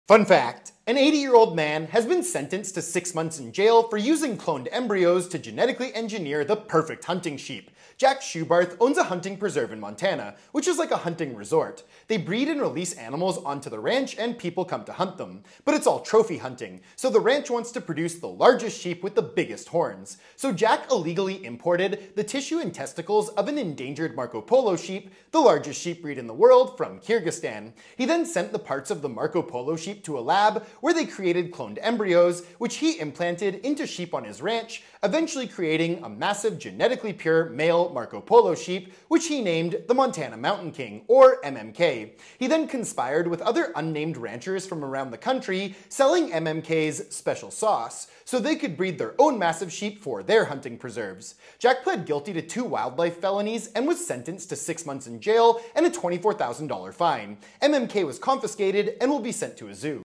80 Year Old Man Goes To Jail For Genetically Engineering Hunting Sheep
An 80 year old man has been sentenced to 6 months in jail for using cloned embryo’s to genetically engineer the perfect hunting sheep.